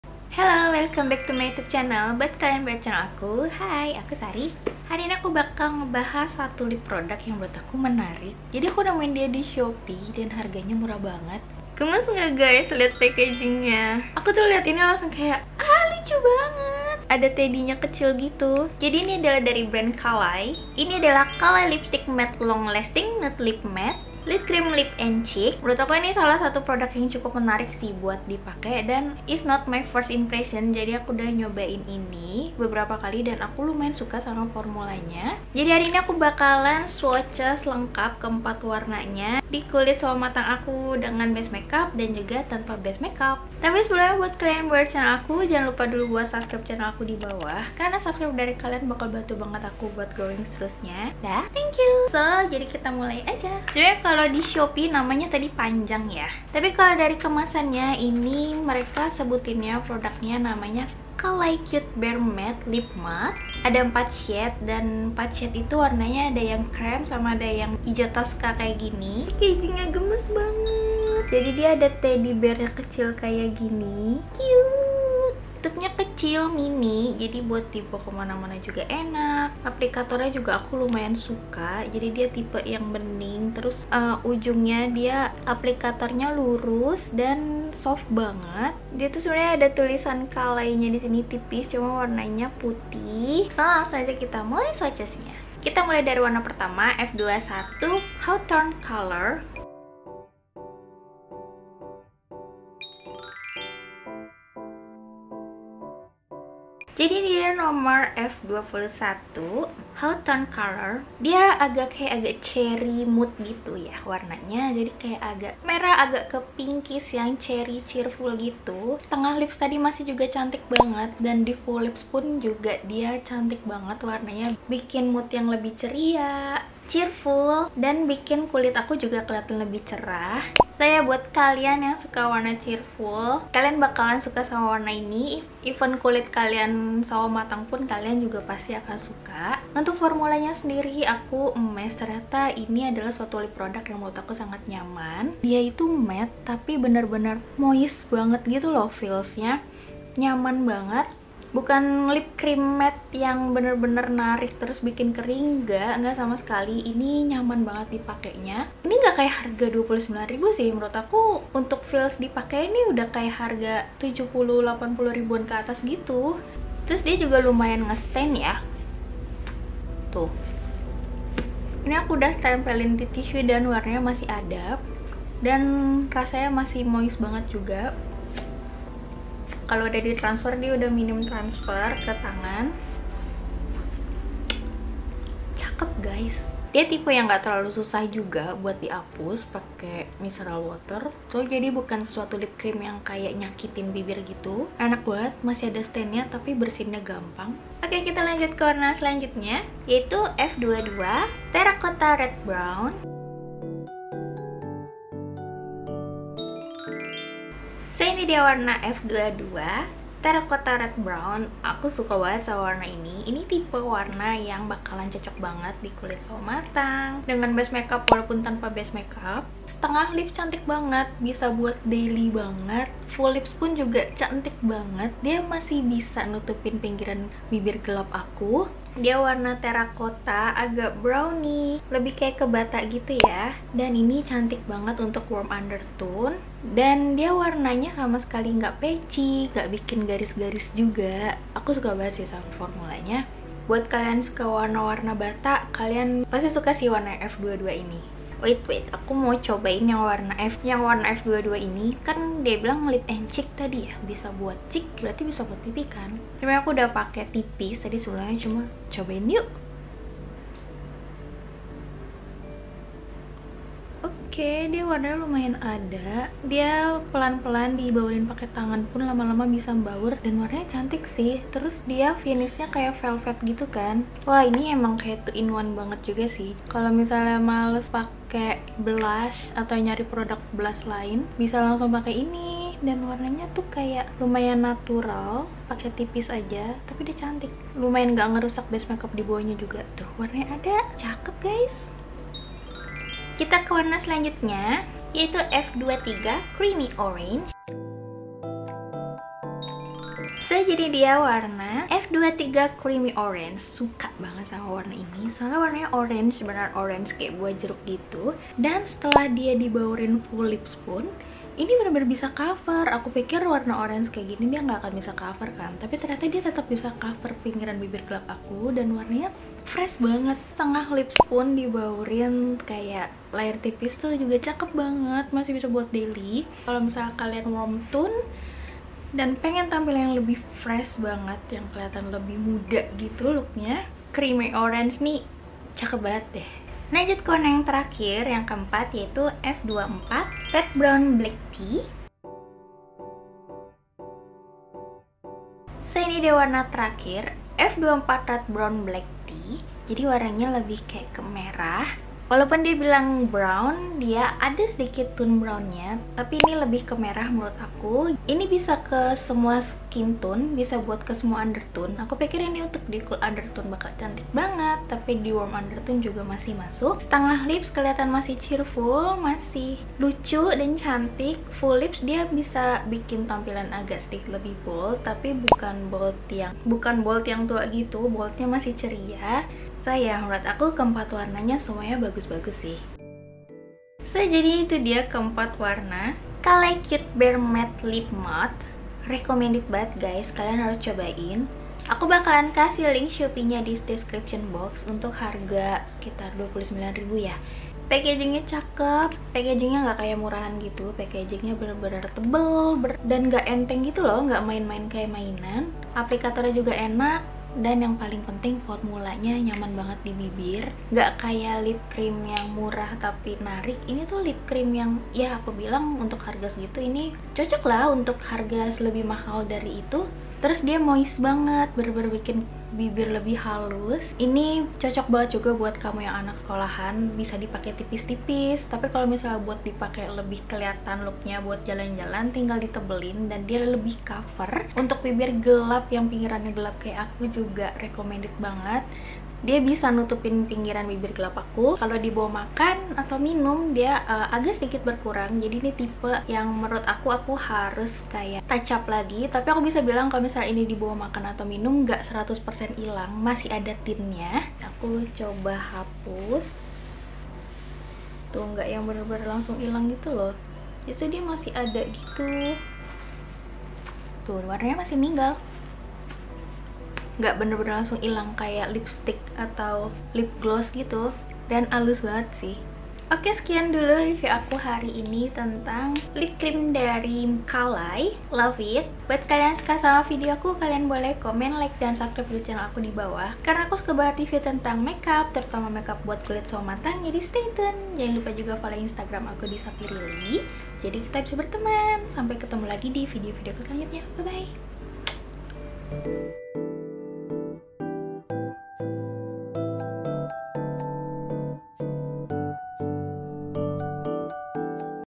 Lip Cream Murah tapi Sebagus ini ! CALAY CUTE BEAR MATTE LIP MUD Swatches di kulit Sawo Matang
hi guys, hari ini aku nemuin lip cream yang menurut aku bagus dengan harga yang sangat murah dan terjangkau cuma 20ribuan

mereka tersedia dalam 4 shades yang menurutku semuanya cantik dan cocok juga untuk kulit sawo matang
eh ini cocook untuk semua skintone sih kurasa dan warnanya juga daily
dan untuk formulanya aku suka banget sih, murah tapi sama sekali ga bikin kering dan nyaman banget di bibir

ini link toko shopee nya yah kalau kalian mau kepoin produk lainnya